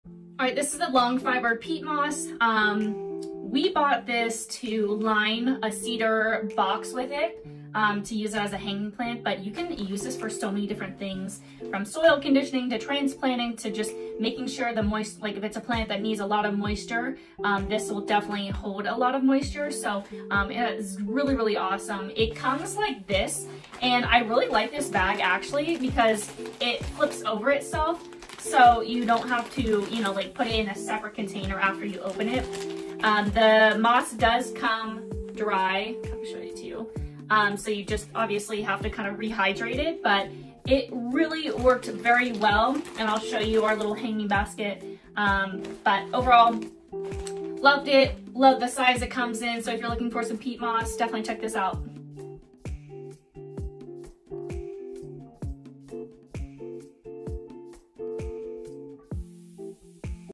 What to know about this Sphagnum Moss from Amazon!
Love using this stuff for my plants to hold the moisture or act as a barrier for hanging plants!